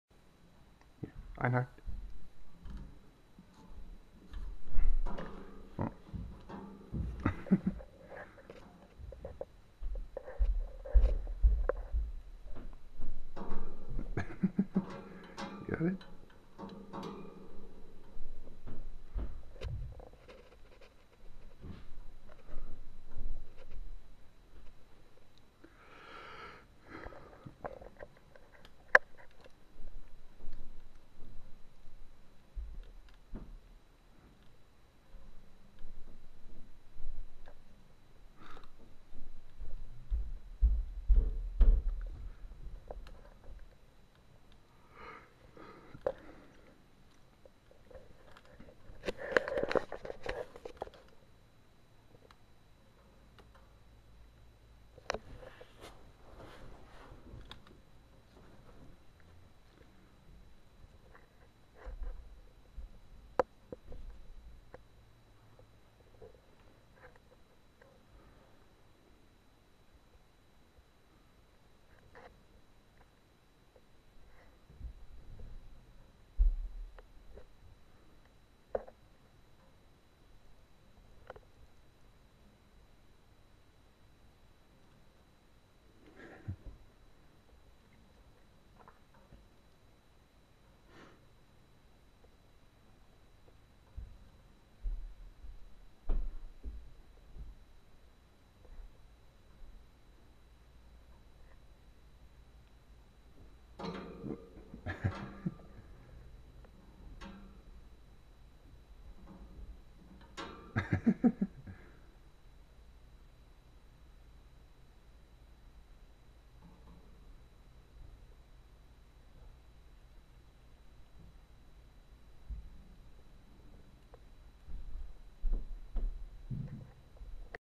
Einhart's Yogurt Chip
I had expected her to stuff whatever she didn't eat. The next time I give Einhart or Vivio a Yogurt chip, I'll be sure to split one chip between them.